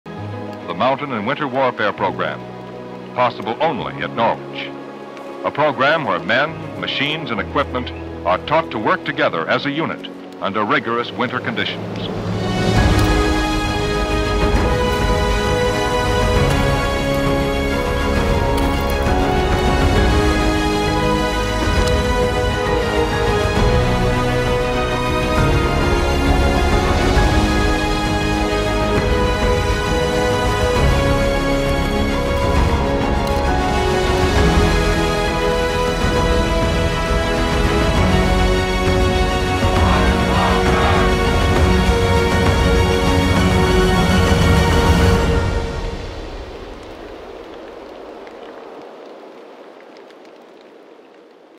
Mountain Cold Weather | Step Outside Your Comfort Zone
Dare to step outside your comfort zone at Norwich University's Mountain Cold Weather Battalion.

Founded in 1947 and once titled the Mountain and Winter Warfare Program, MCW has grown and evolved over time. However, what hasn't changed is our commitment to training cadets to reach their potential as mountain combat soldiers using military mountaineering techniques and small-unit patrolling tactics.

Experience unparalleled growth only here. Are you ready for it?
It all starts with our motto, “I Will Try.” Discover a community that believes trying is an act of bravery and learning comes from struggling. Here, success means becoming a better version of yourself, capable of going beyond what you thought possible.